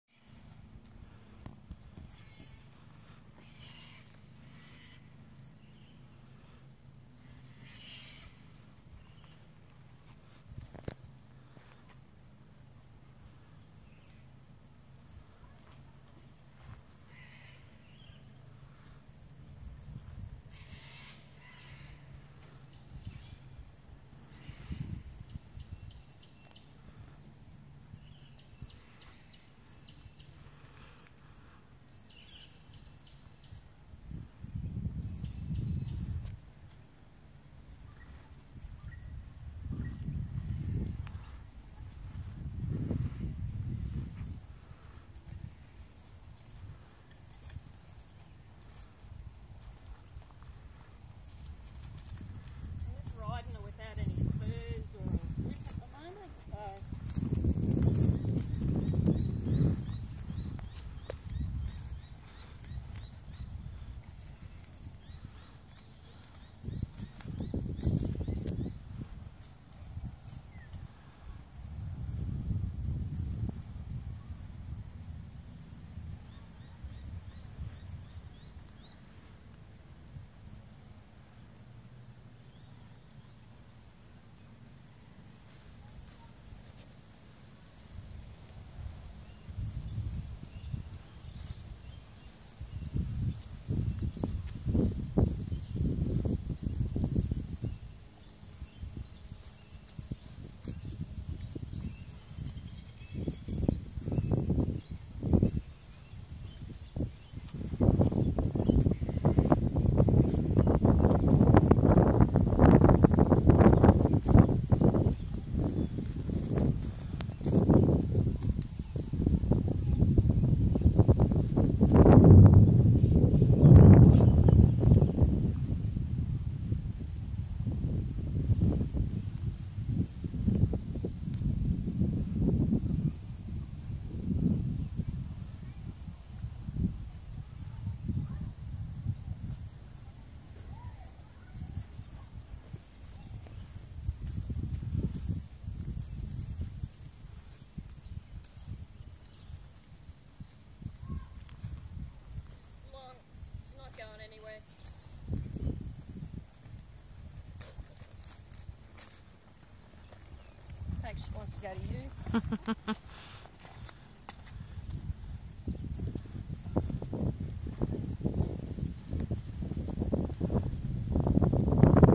Stella Connemara Pony
At the walk - have just hoped on her, she has not been ridden for 3 or 4 weeks.